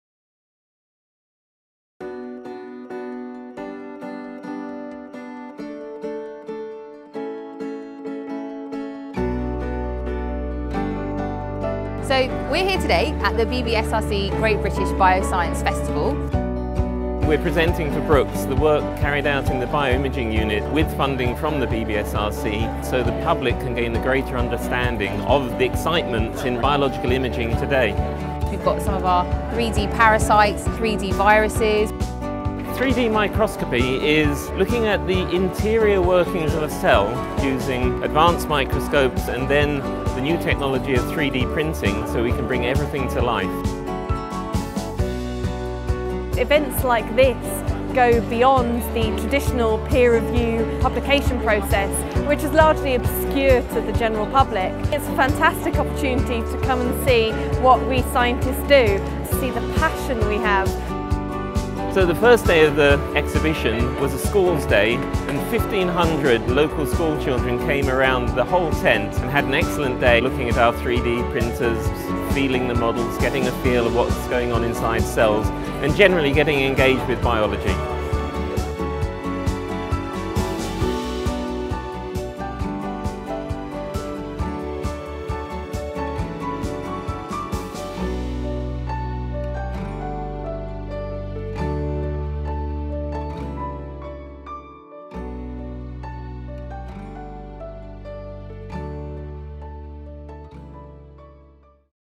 Miniature to Massive
3D models of Cells and Viruses from the Bioimaging Unit at Oxford Brookes are shown at the Biotechnology and Biosciences Research Council (BBSRC) Great British Bioscience Festival.